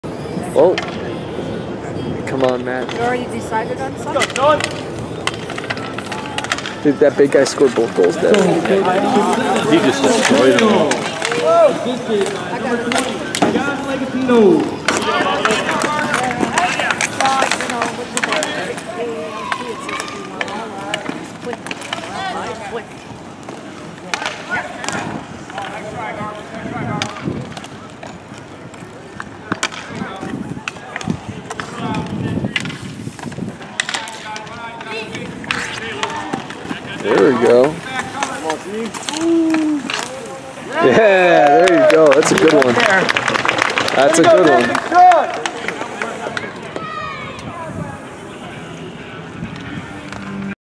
I scored the first goal in the 2018 Roller Hockey High School Championship game I’m number 97
We were down 2 to 0 and I scored the first goal to give us some momentum